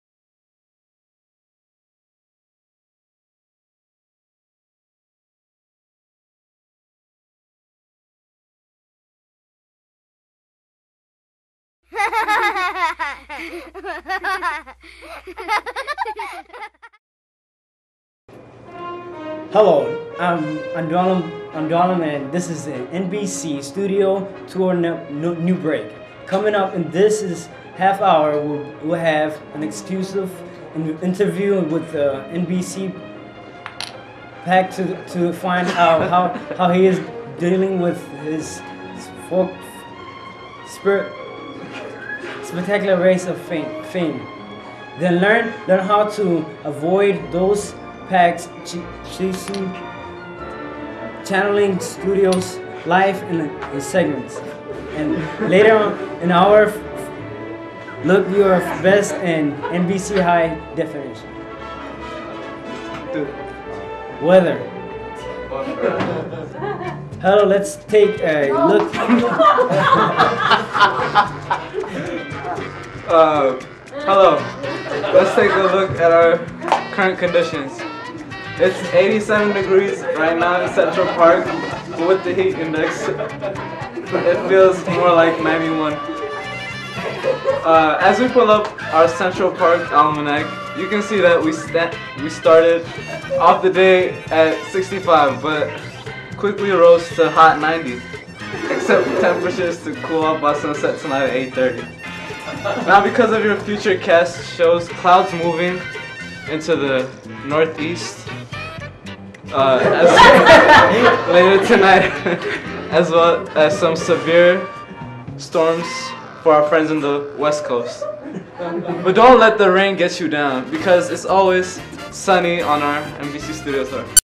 Biggest fail at NBC News brought to you by Andu
My bro Andu a.k.a Andy forgot to put on his contacts when he visited the NBC news studio. Since he wasn't able to see clearly, he misunderstood all the words that he had to reads so he made up his own words. Watch Andu and his friend fail so bad at giving live news.